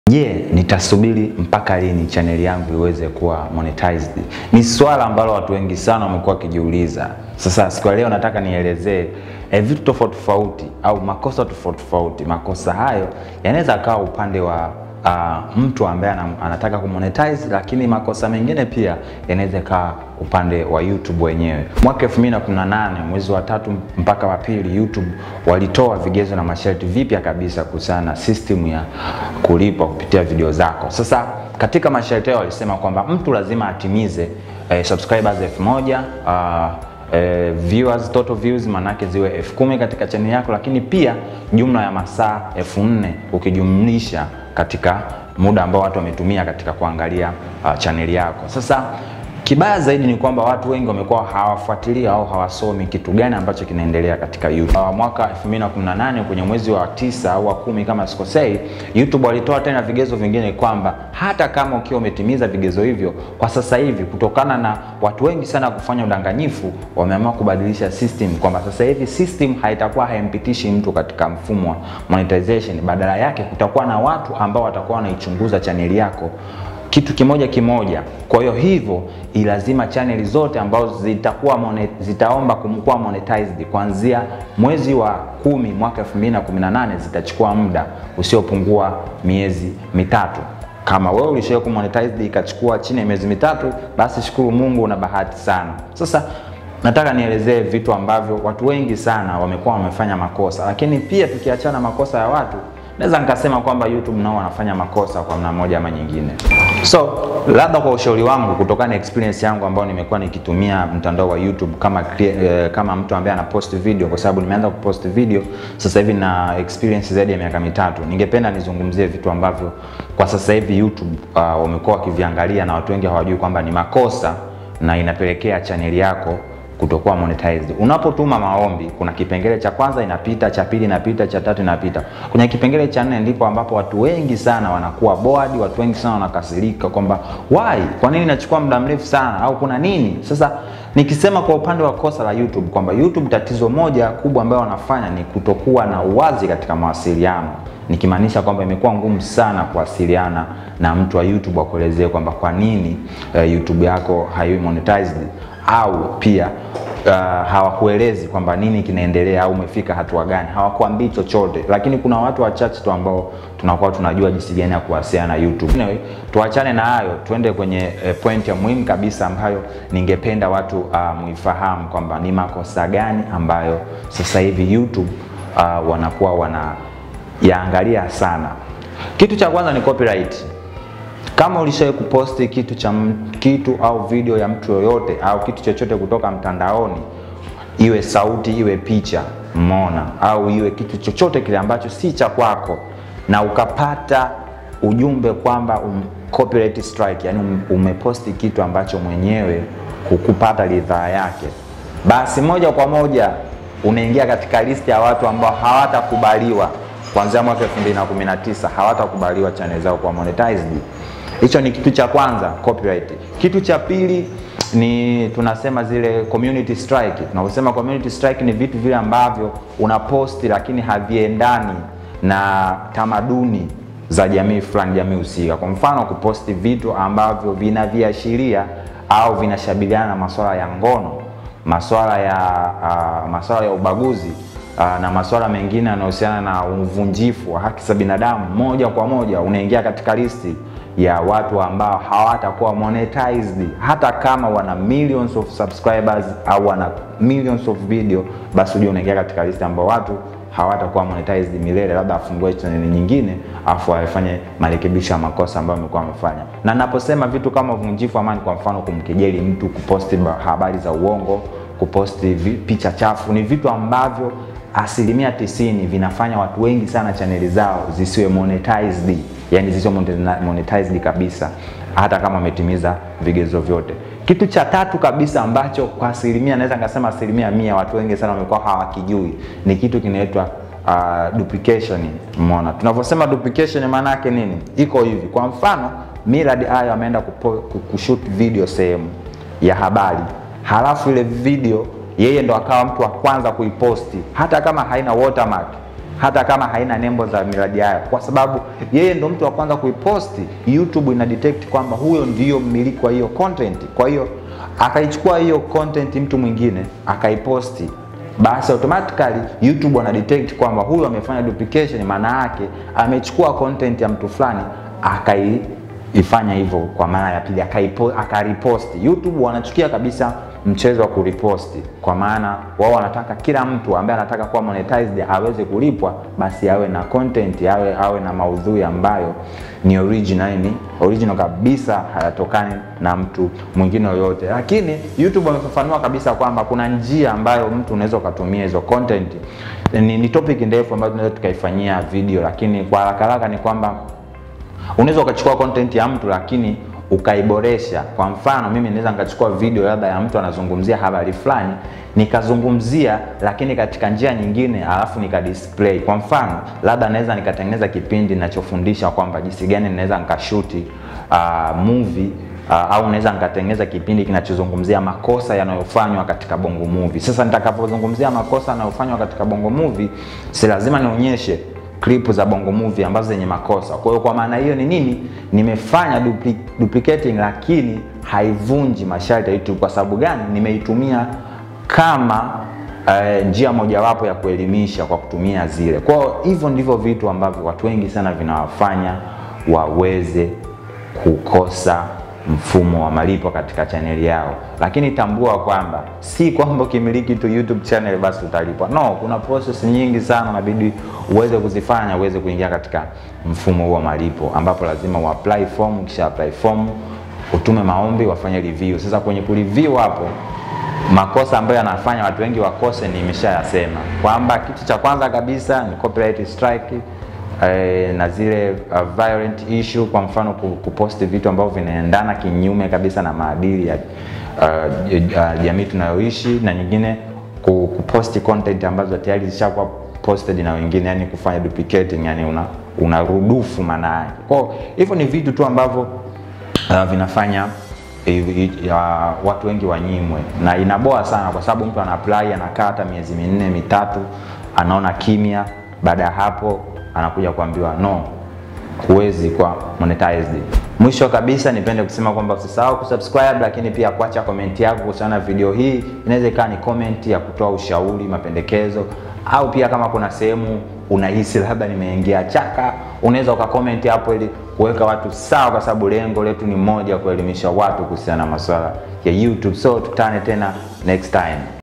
YouTube Monetization, Nitasubiri kwa mda gani kukubaliwa ?
Video hii inafafanua kuhusu kukubaliwa na mfumo wa youtube partner ili uanze kupokea malipo kutokana na Video zako.
 Ndio YouTube wanalipa watu ambao wanapost Video zao na kuruhusu matangazo yao yapite katika Video zao.